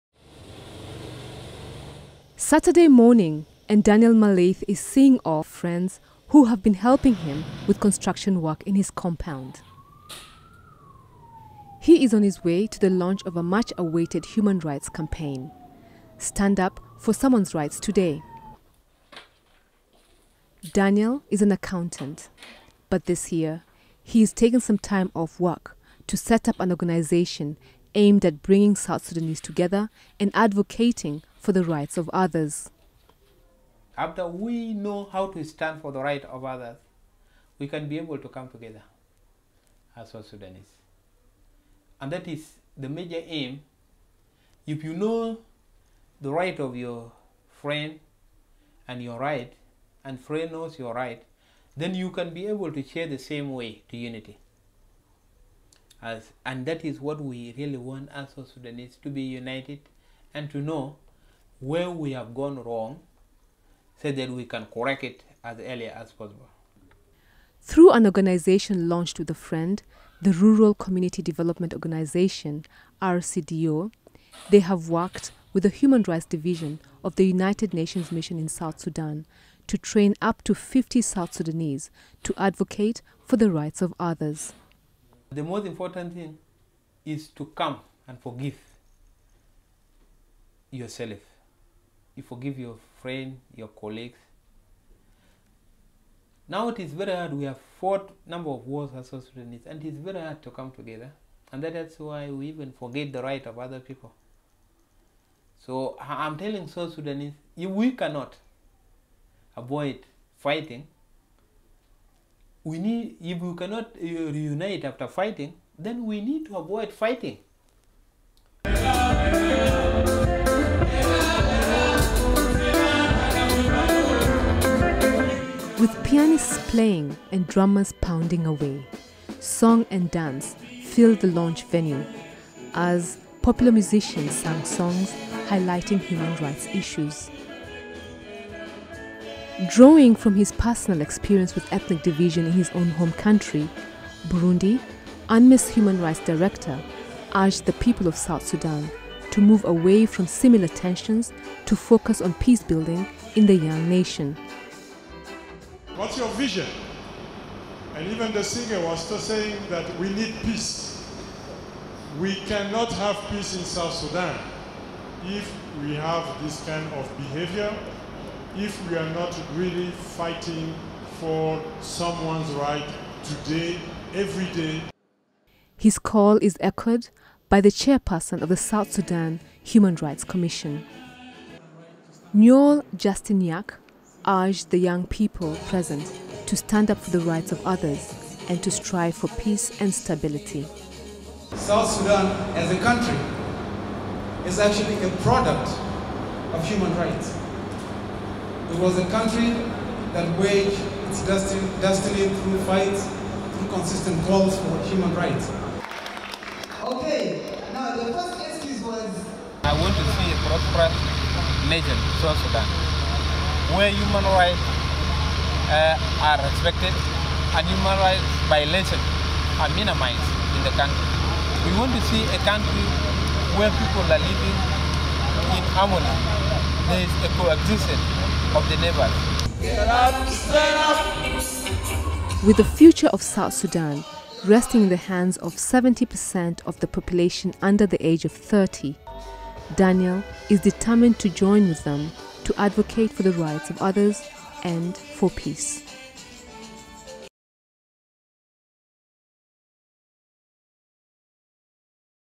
Stand Up for Someone's Rights Today
The United Nations Mission in South Sudan (UNMISS) launched the “Stand Up for Someone’s Rights Today” human rights campaign.